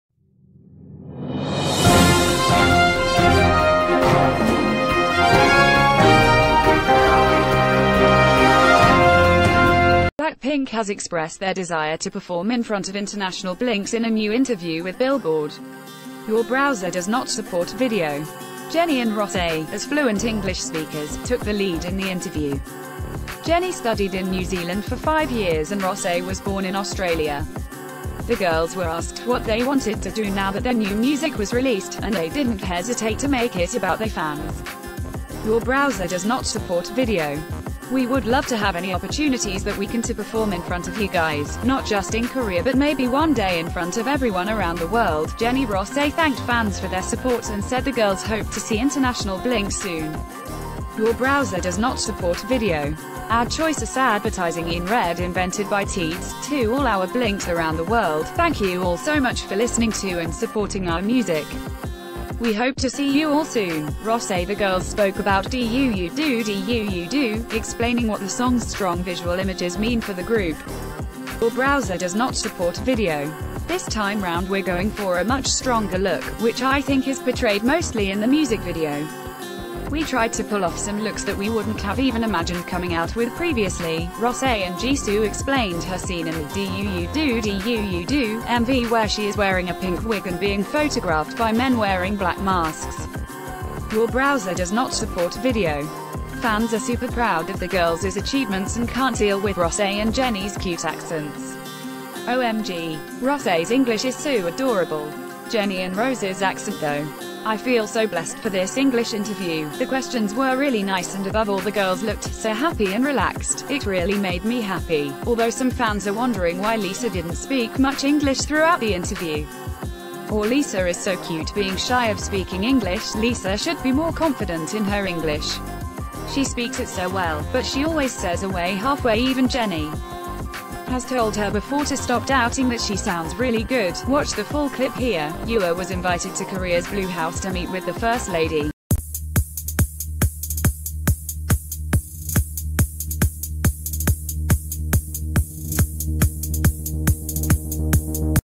BLACKPINK Holds Another English Interview, Says They Want To Perform In Front Of International Fans
BLACKPINK has expressed their desire to perform in front of international BLINKs in a new interview ...